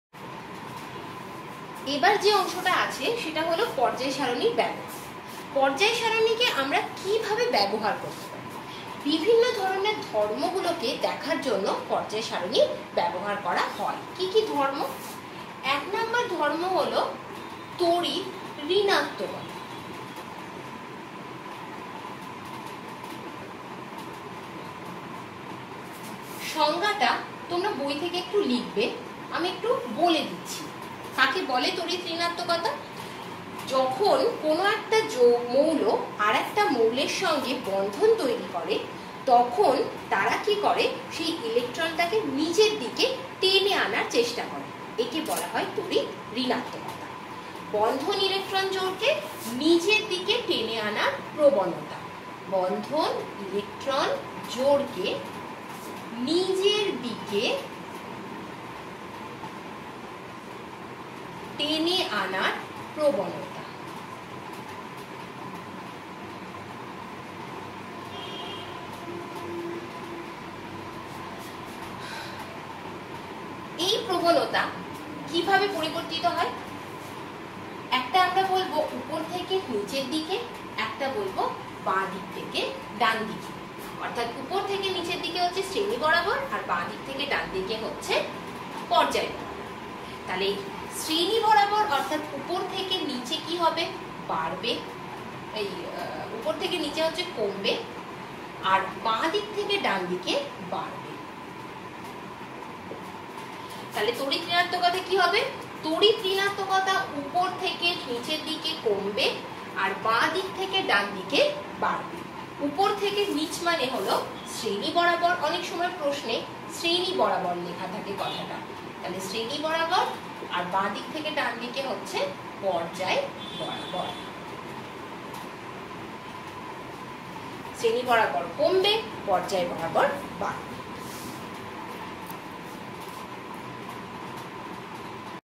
use of periodic table
how do we use the knowledge of periodic table is described here